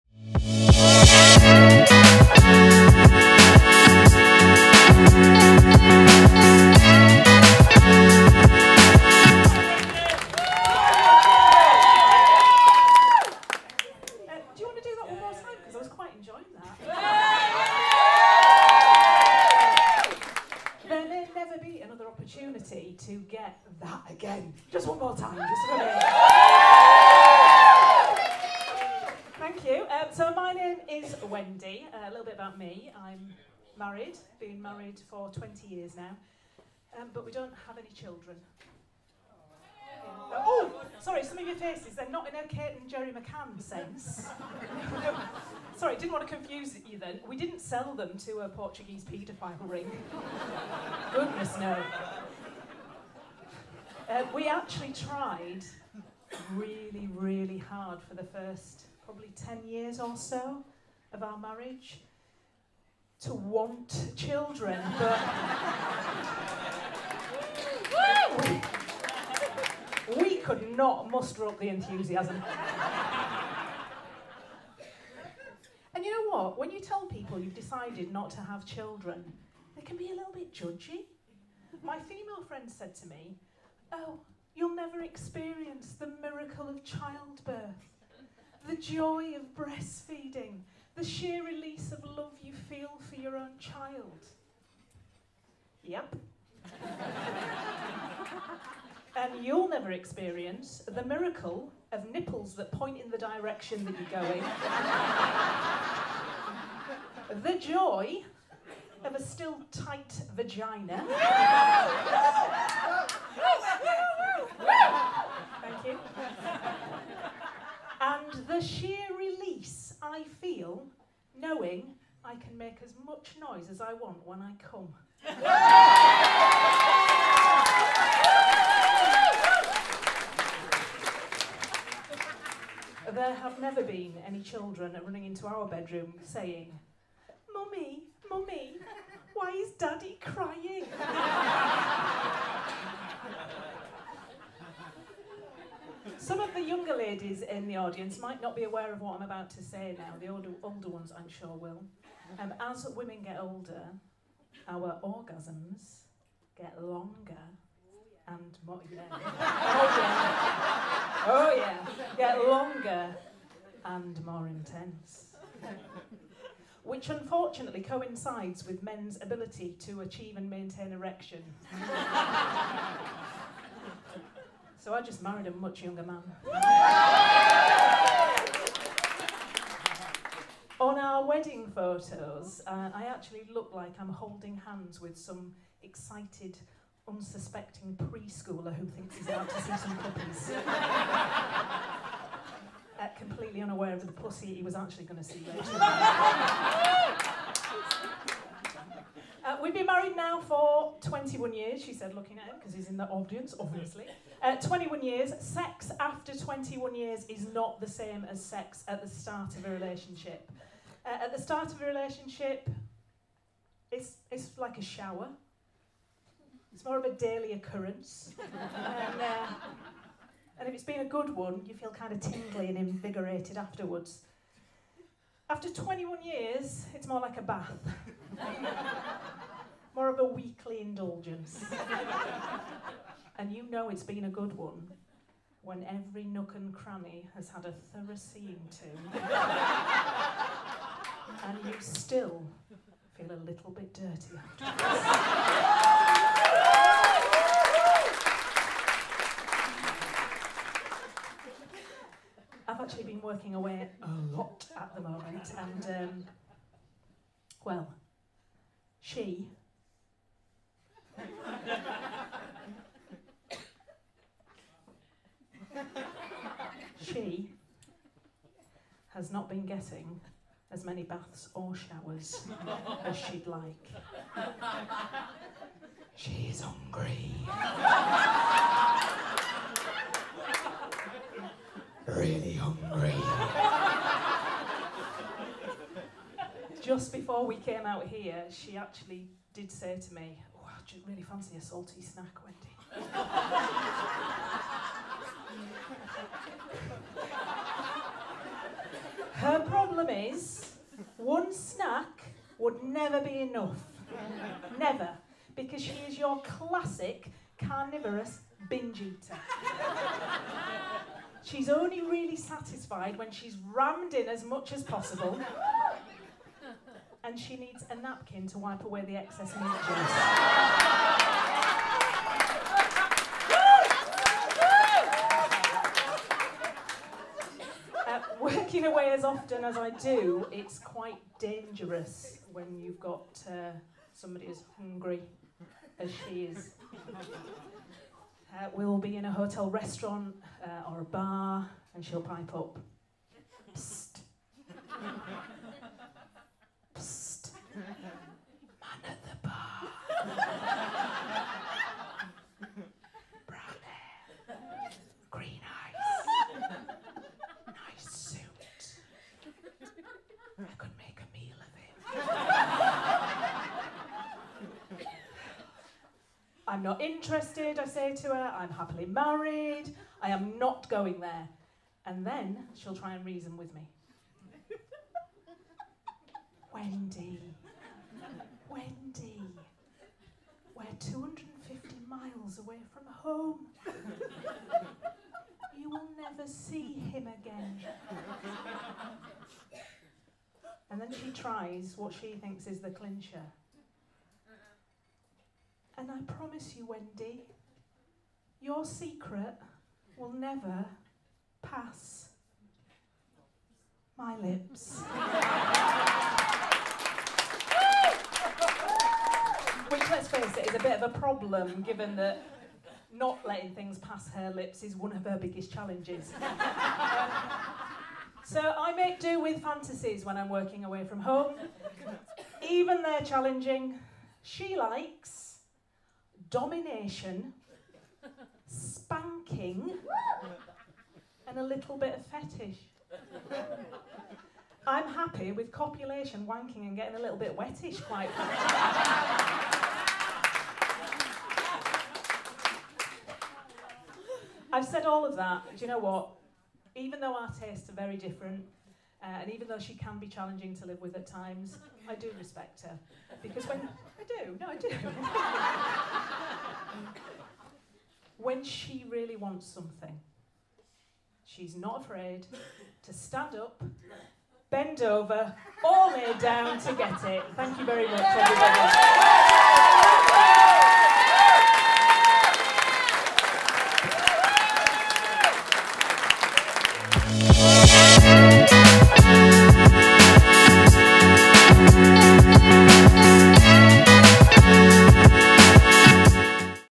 LEEDS | ULTRA COMEDY | WENDY WOO
Master the art of stand-up comedy with 8 weeks FREE coaching!

Participants taking part in Ultra Comedy have had 8 weeks of FREE coaching from a professional comic to stand up and make friends and family laugh at a comedy event!

• Learn the art of stand up with 8 weeks free coaching
• Stand up and some of the UK's biggest comedy clubs!

Want to raise money for Cancer Research UK?